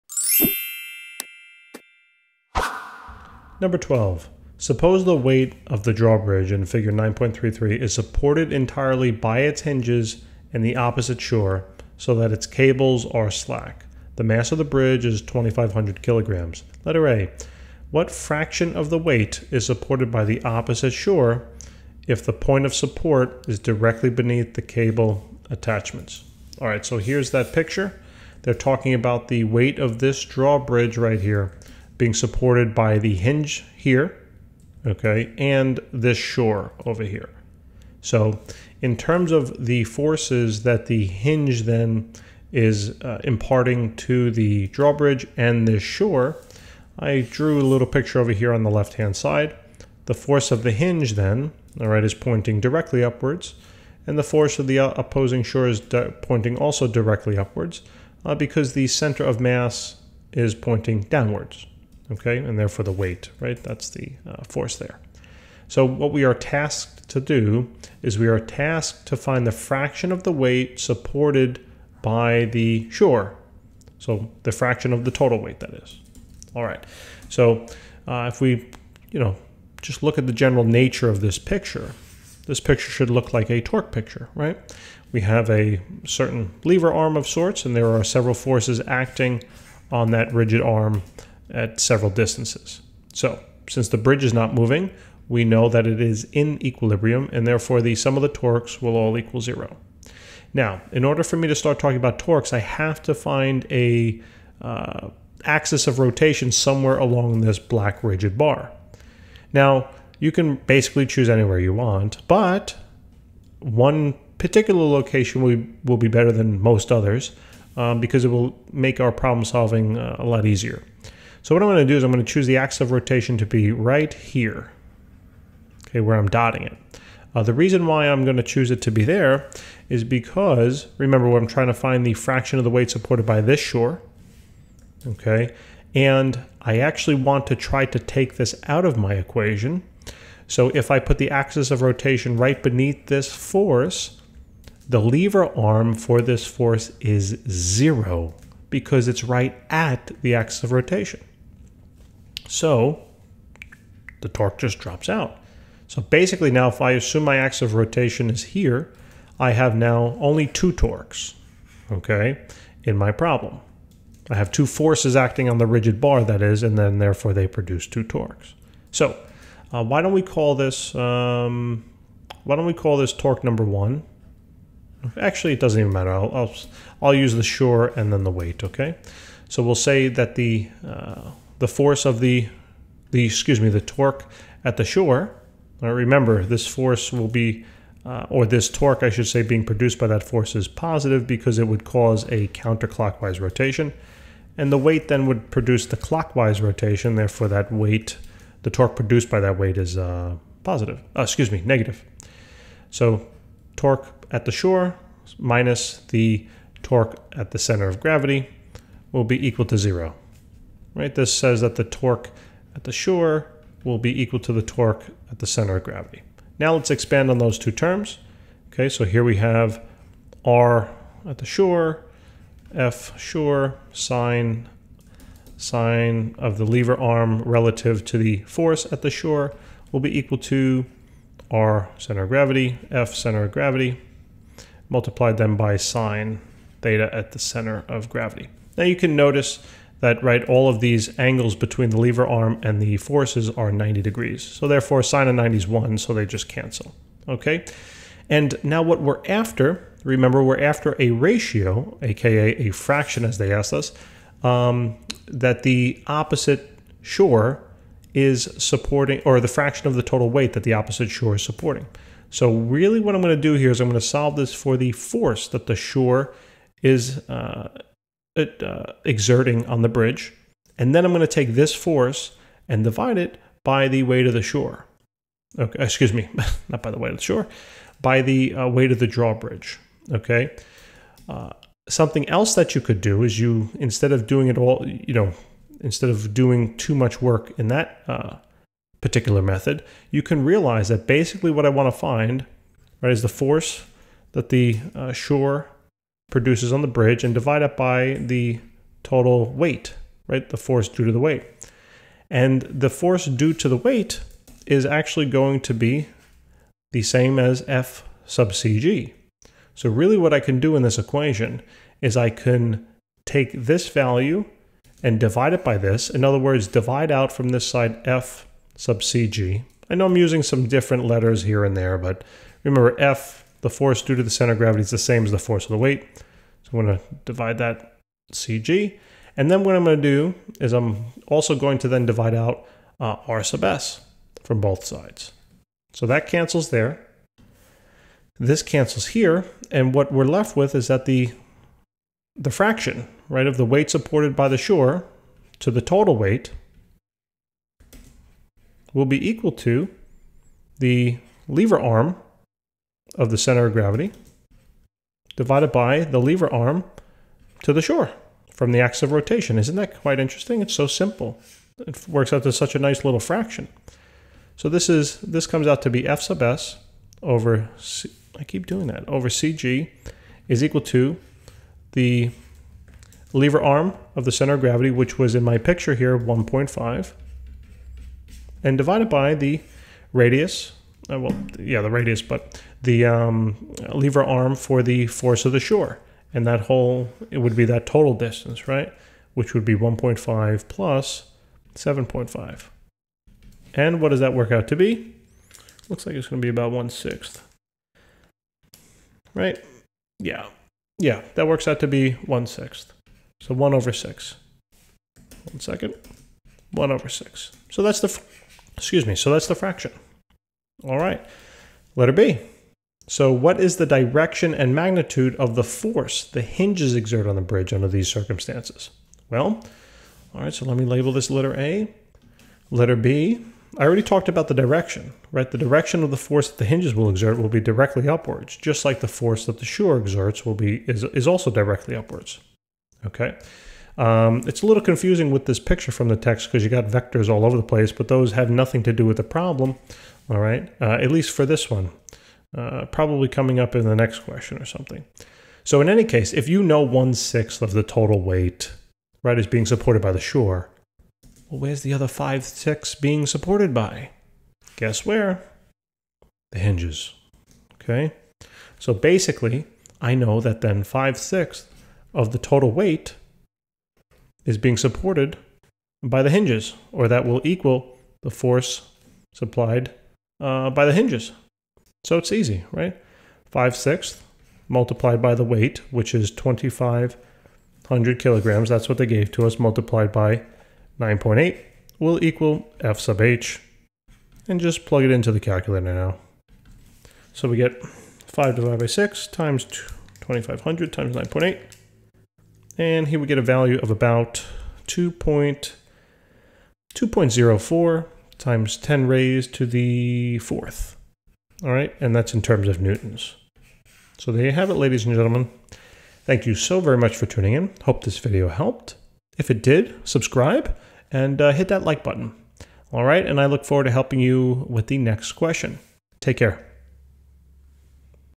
9.12 | Suppose the weight of the drawbridge in Figure 9.33 is supported entirely by its hinges and
Suppose the weight of the drawbridge in Figure 9.33 is supported entirely by its hinges and the opposite shore, so that its cables are slack. The mass of the bridge is 2500 kg. (a) What fraction of the weight is supported by the opposite shore if the point of support is directly beneath the cable attachments? (b) What is the direction and magnitude of the force the hinges exert on the bridge under these circumstances?